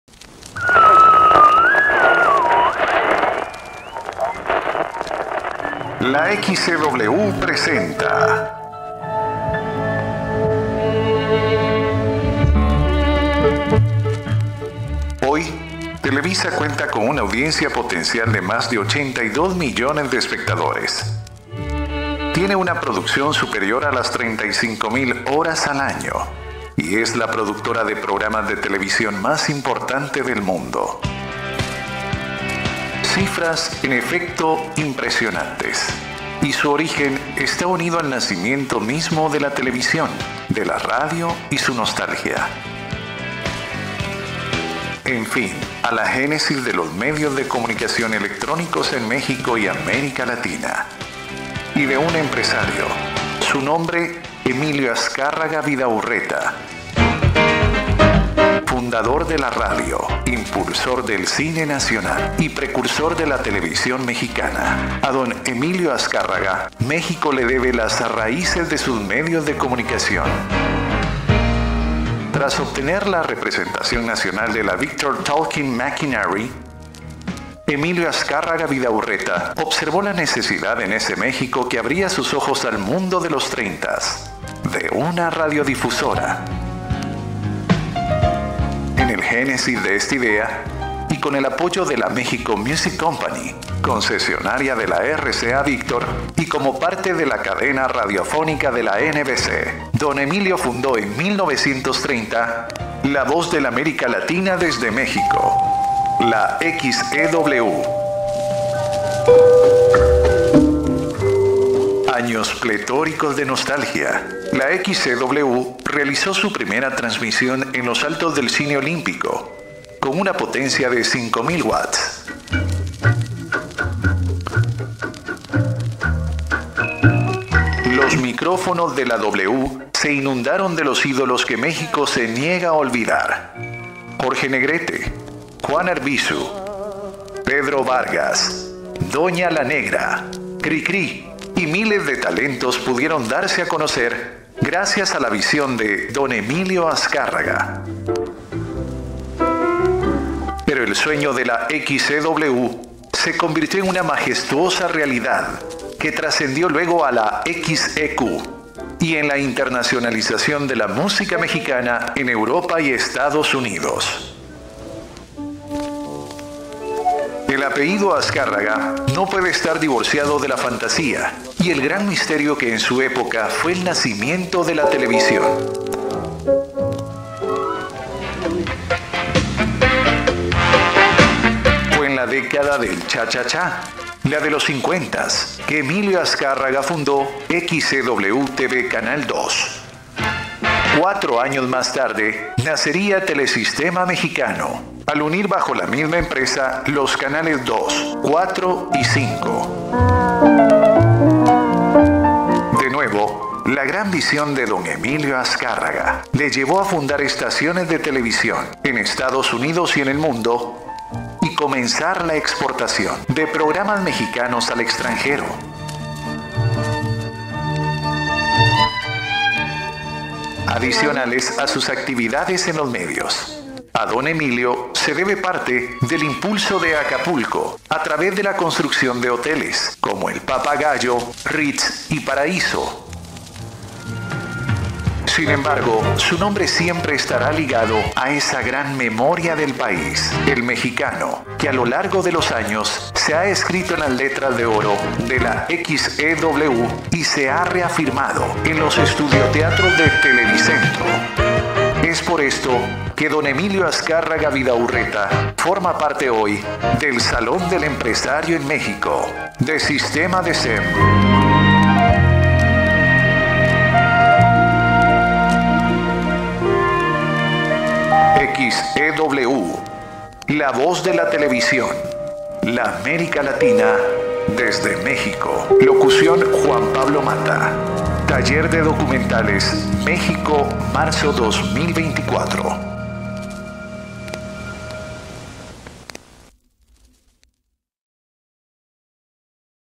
Historia de Televisa
Para fundar la XEW, Emilio Azcárraga Vidaurreta, entonces presidente de la México Music Company, obtuvo un crédito del Banco de Londres y México